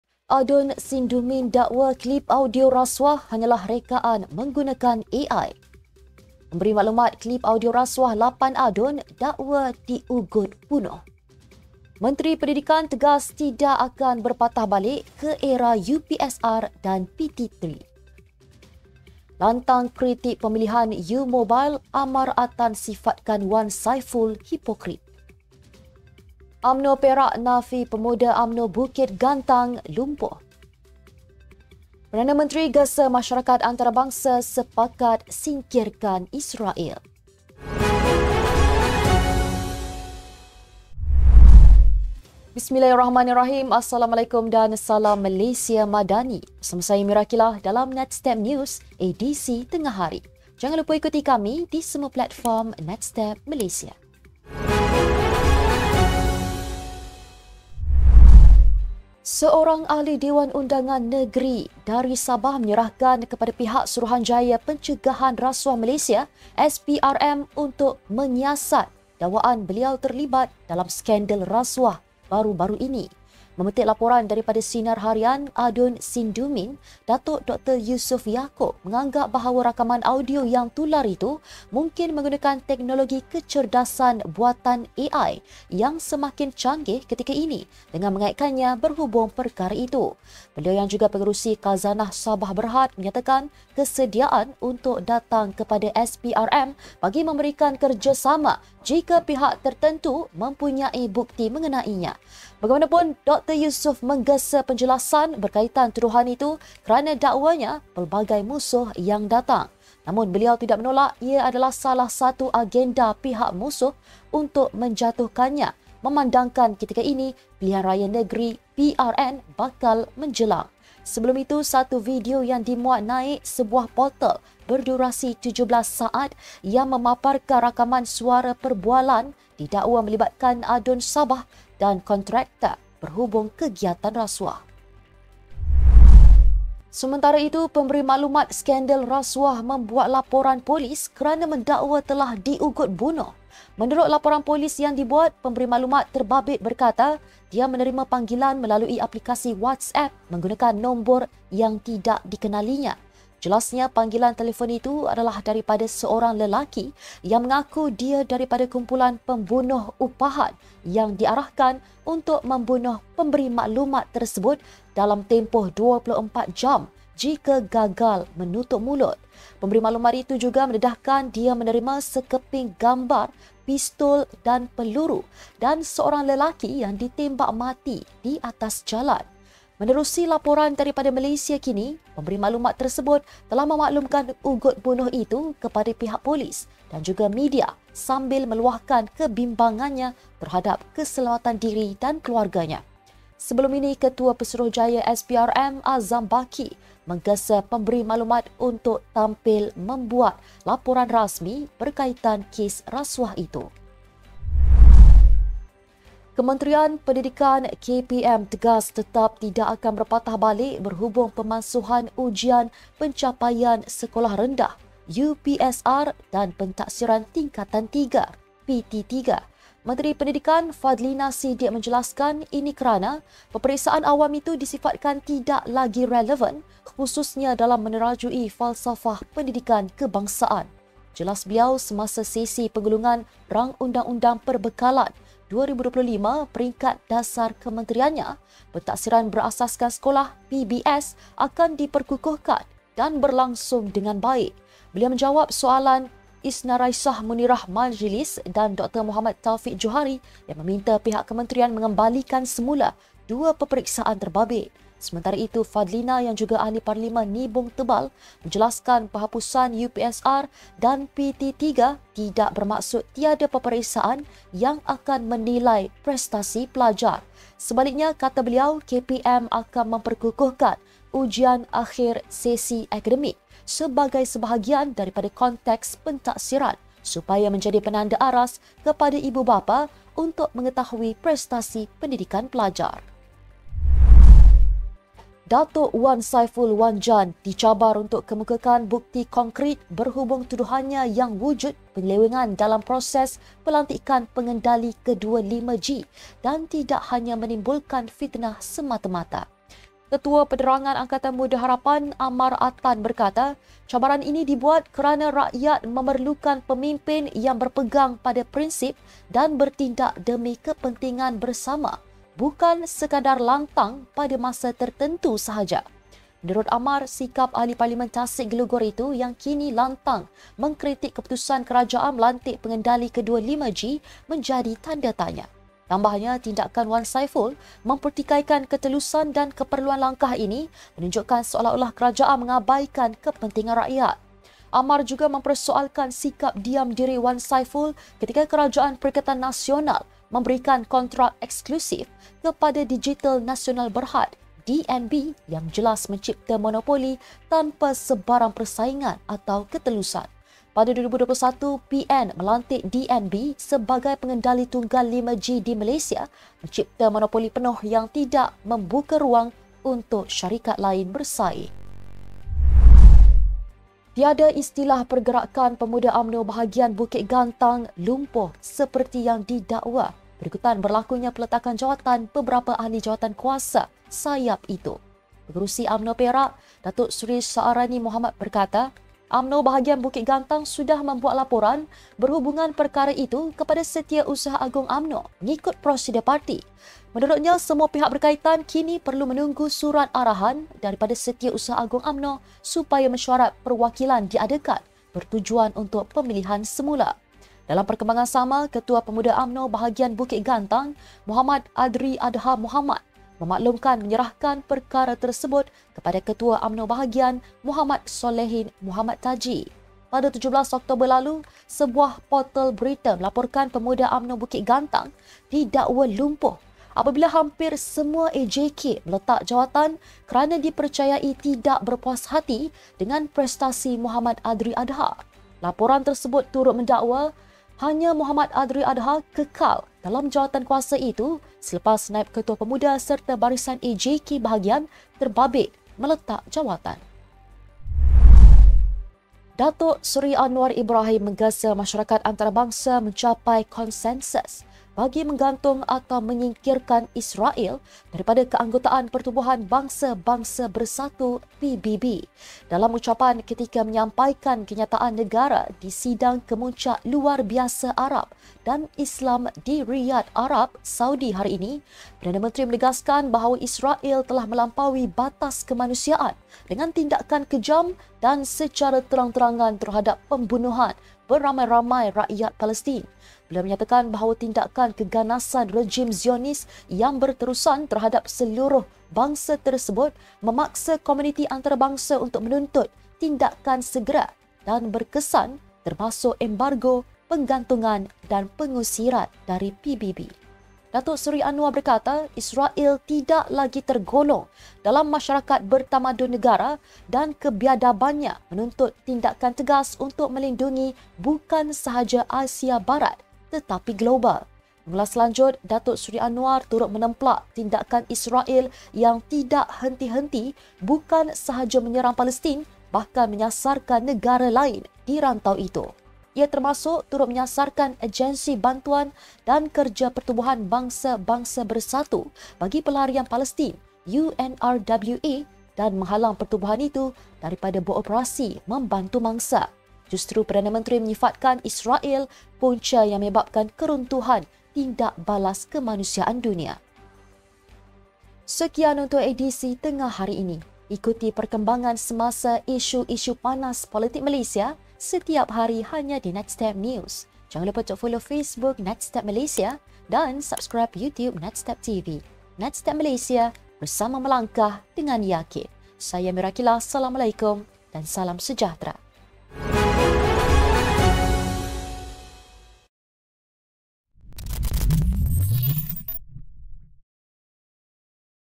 TERKINI! Adun GRS 'dipetik' terkait Adun rasuah? | Kontrak 5G: Wan Saiful hipokrit- PH | UPSR & PT3
Antara menarik tengah hari ini:

1. Adun Sindumin dakwa klip audio rasuah guna Ai, serah SPRM siasat kes
2. Pemberi maklumat klip audio rasuah 8 Adun dakwa diugut bunuh
3. Menteri Pendidikan tegas tidak akan berpatah balik ke era UPSR dan PT3
4. "Perkara ini menunjukkan sikap Wan Saiful yang amat hipokrit" - Ammar Atan
5. UMNO Perak nafi Pemuda UMNO Bukit Gantang 'lumpuh'
6. Perdana Menteri gesa masyarakat antarabangsa sepakat singkirkan Israel

Ikuti perkembangan semasa isu-isu panas politik Malaysia, setiap hari, hanya di NextStep Malaysia 🇲🇾

“Bersama Melangkah Dengan Yakin”